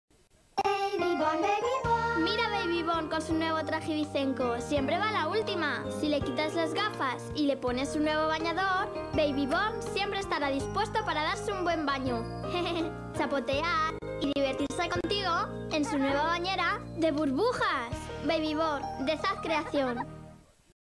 Baby Born en la playa (2006)
Anuncio del muñeco Baby Born y de sus accesorios de playa, fabricados por Zapf Creation. Emitido en 2006.